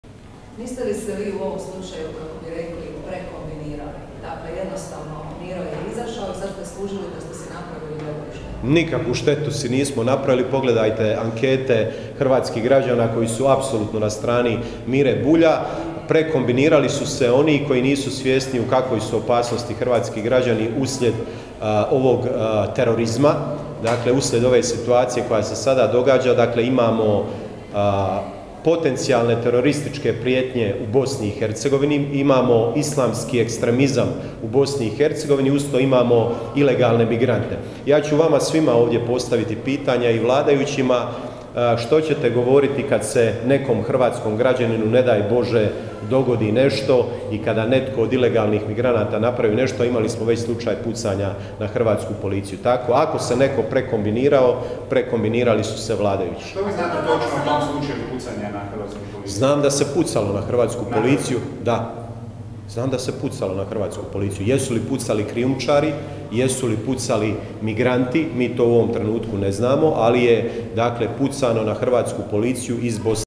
Grmoja: "Nikakvu štetu si nismo napravili"
Na konferenciji za medije održanoj u Saboru o migrantskoj krizi i sjednici odbora za nacionalnu sigurnost govorio je Nikola Grmoja.